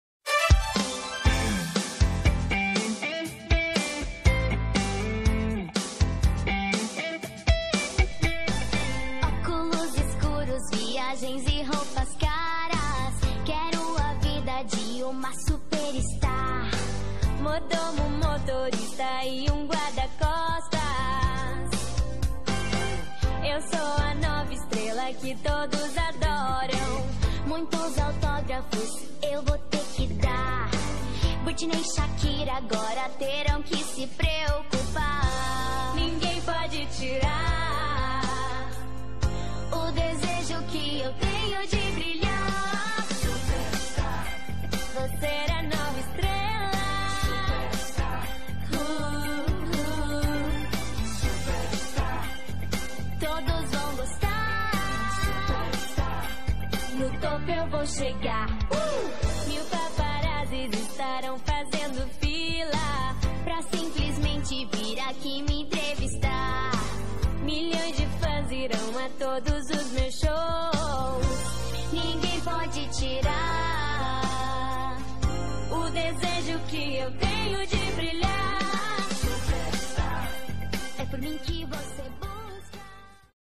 Fotos da Isabela Junqueira! C1R!!😋
Nesse video mostramos fotos da atriz Larissa Manuela que faz duas personagens Isabela e Manuela espero que gostem super beijo!!!😚😚